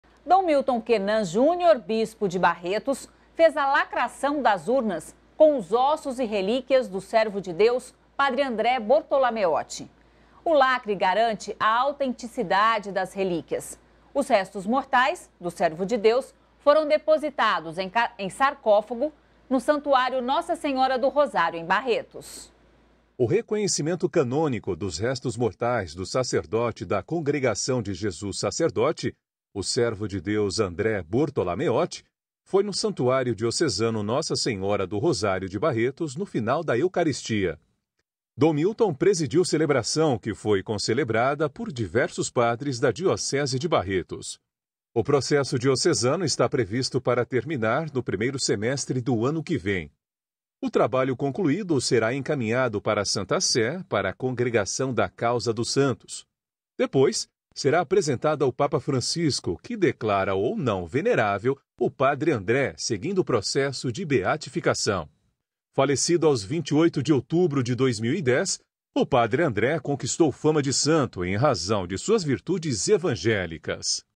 Urna com restos mortais do Servo de Deus Padre André Bortolameotti é lacrada - JCTV - 02/12/21
Dom Milton Kenan Júnior, bispo de Barretos fez a lacração das urnas com os ossos e relíquias do Servo de Deus padre André Bortolameotti. O lacre garante a autenticidade das relíquias. Os Restos mortais do Servo de Deus foram depositados em sarcófago no Santuário Nossa Senhora do Rosário em Barretos.